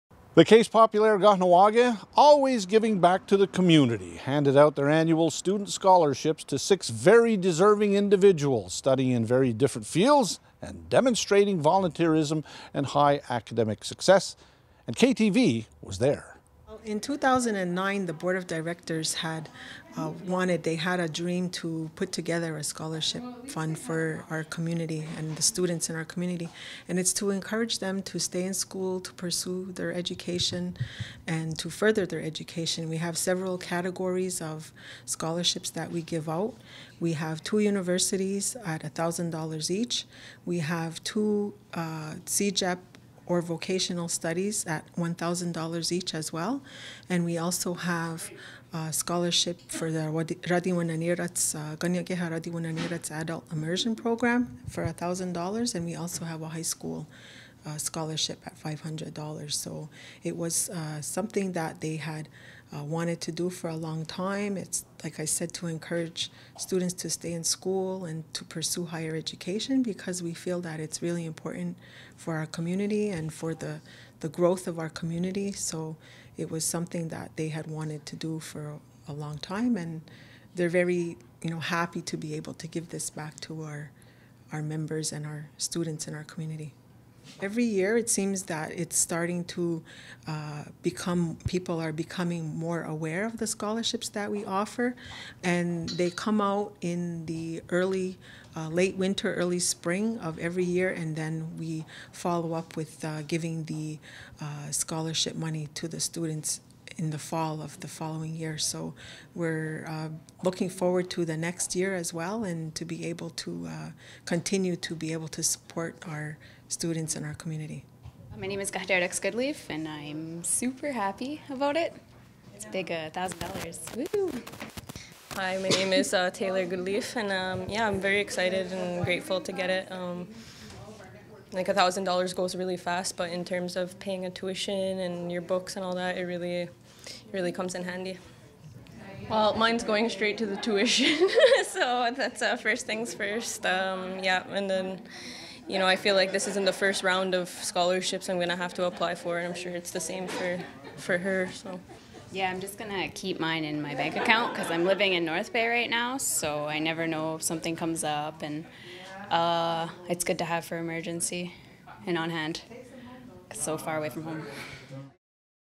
Caisse Populaire Kahnawà:ke awards Scholarships to local students
The Caisse Populaire Kahnawà:ke hosted an event to provide several  local students with financial assistance in their education. KTV was on hand and spoke with  general manager Mandy Montour about the donations.